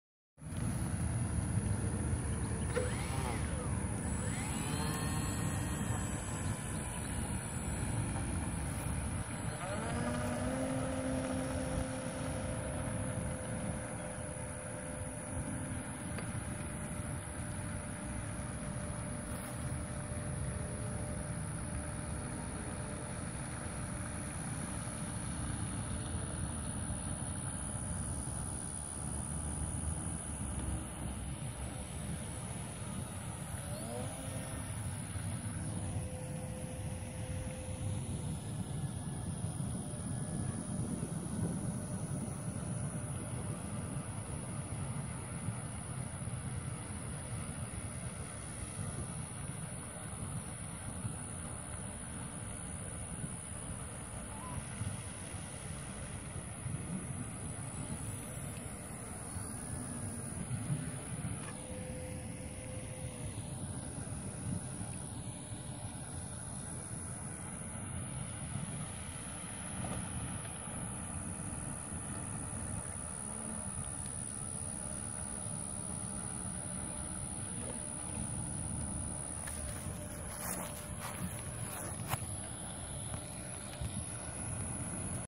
Sportsman with floats
Filmed it woth phone strapped with rubber bands to my transmitter so not best filming but it does work.